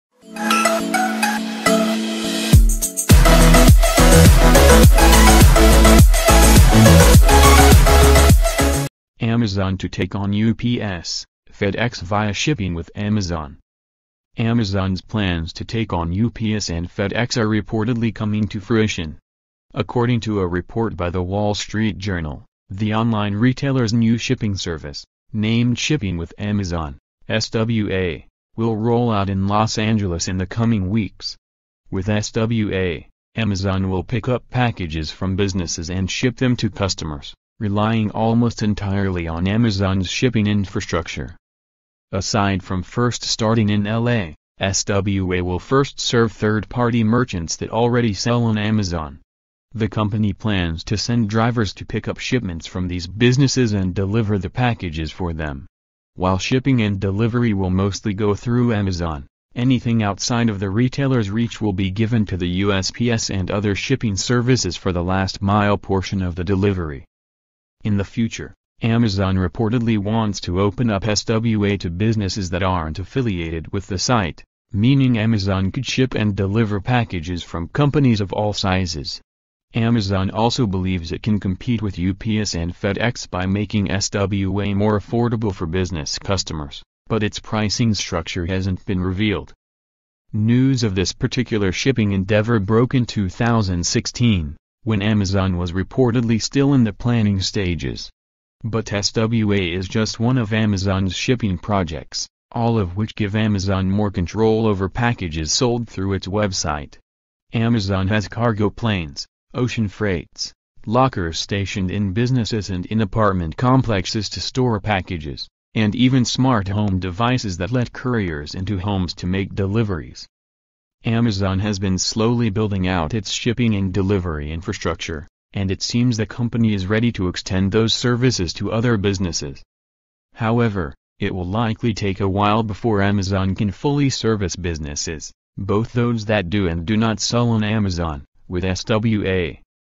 Amazon to take on UPS, FedEx via “Shipping with Amazon”
Amazon's plans to take on UPS and FedEx are reportedly coming to fruition. According to a report by The Wall Street Journal, the online retailer's new shipping service, named "Shipping with Amazon" (SWA), will roll out in Los Angeles in the coming weeks. With SWA, Amazon will pick up packages from businesses and ship them to customers, relying almost entirely on Amazon's shipping infrastructure.

Aside from first starting in LA, SWA will first serve third-party merchants that already sell on Amazon. The company plans to send drivers to pick up shipments from these businesses and deliver the packages for them. While shipping and delivery will mostly go through Amazon, anything outside of the retailer's reach will be given to the USPS and other shipping services for the "last mile" portion of the delivery.